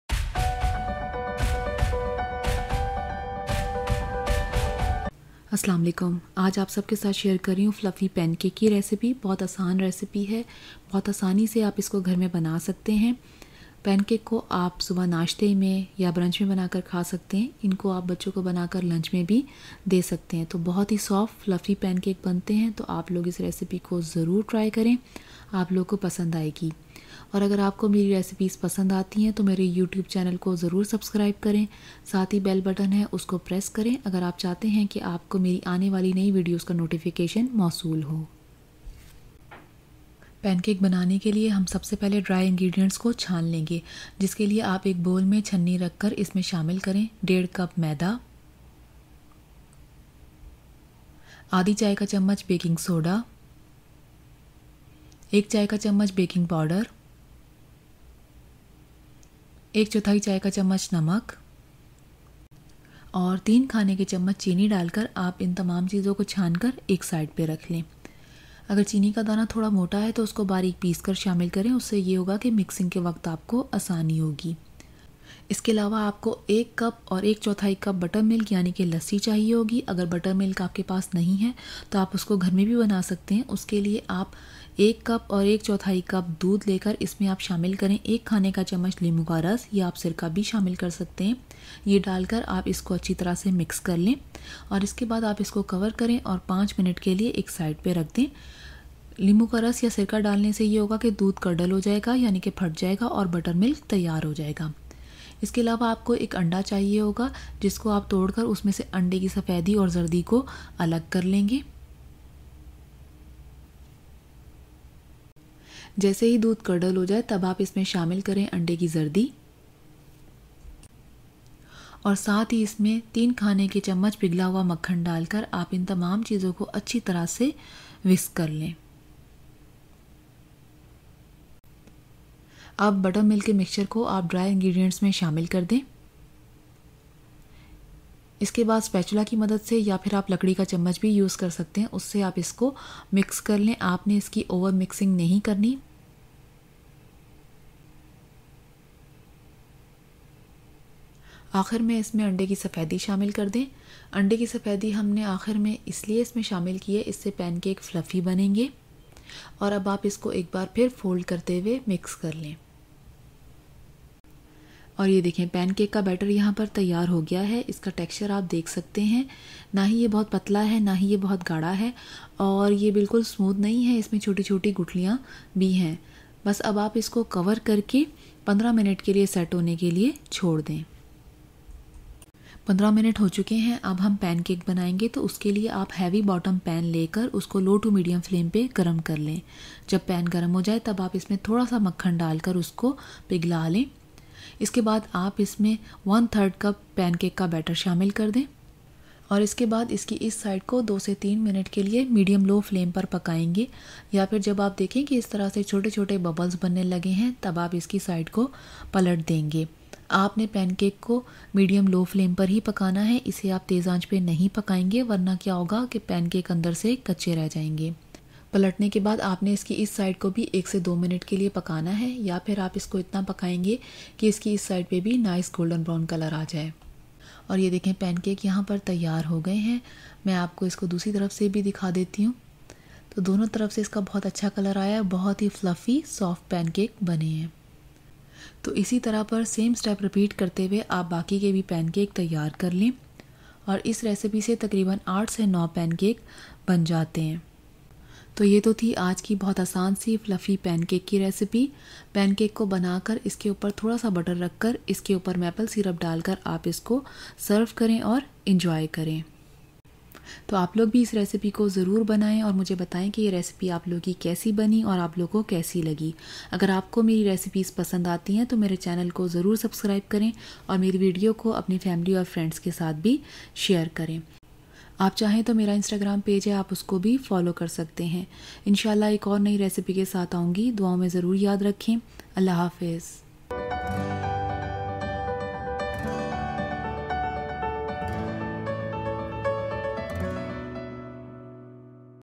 Pancake Recipe - How to Make Fluffy Pancakes from scratch - Breakfast Recipe
Learn how to make Fluffy Pancakes at home. A type of cake, with few ingredients and easy to make. A Step by Step Complete Instruction about Basic Pancake with Quick and Easy Method. A Best Breakfast Pancakes recipe from scratch. Serve hot with accompaniments of your choice like, butter, margarine, honey or maple syrup.
Ingredients
1½ Cups Flour
1 Teaspoons Baking Powder
½ Teaspoons Baking Soda
¼ Teaspoons Salt
3 Tablespoon sugar
1 Egg
1¼ Cups Milk
1 Tablespoon Lemon juice or Vinegar
3 tbsp Melted Butter + more for cooking

I N S T A G R A M:
Madeeha Imtiaz